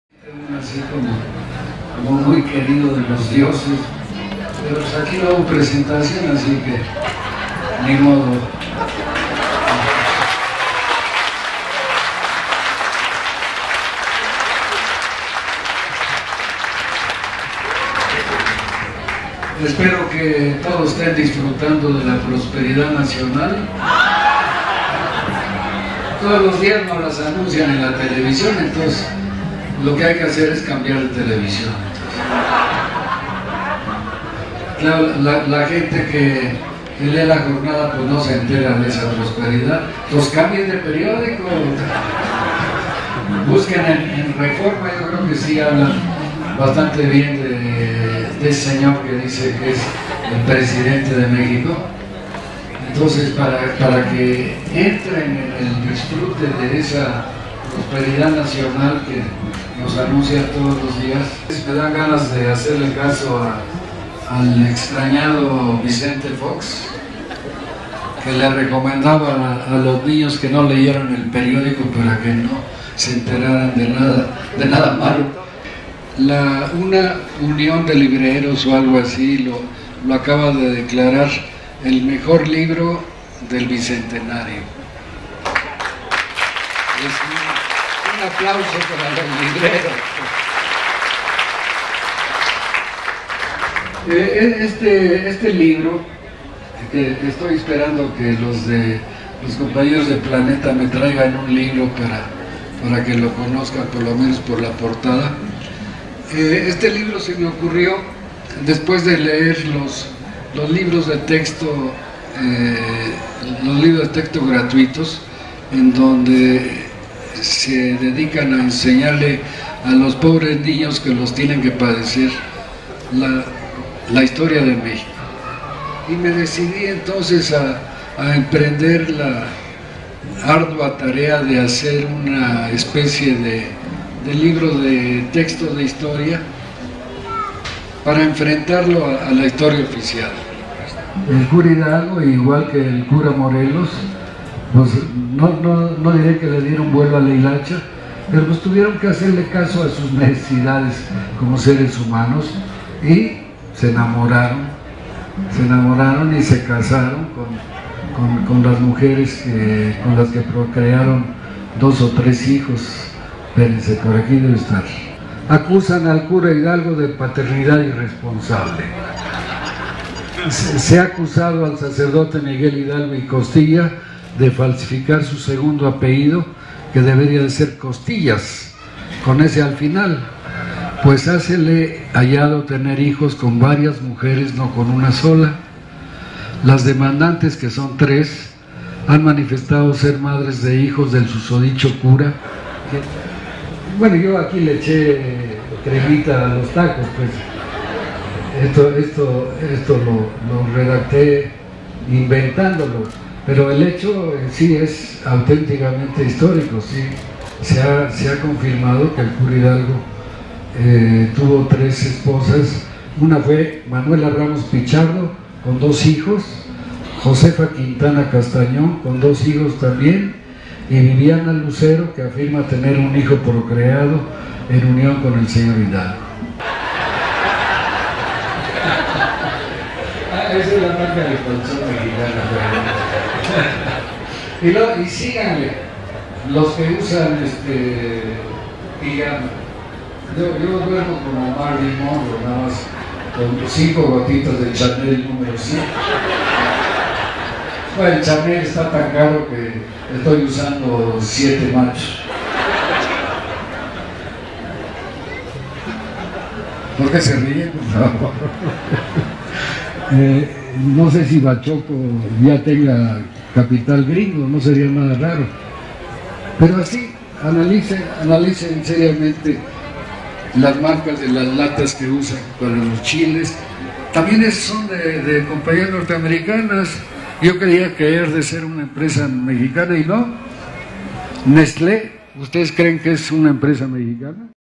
RIUS "Ni libertad ni Independencia"
Rius presenta su libro NI LIBERTAD NI INDEPENDENCIA en el marco de la Feria del libro Alternativa de la Alameda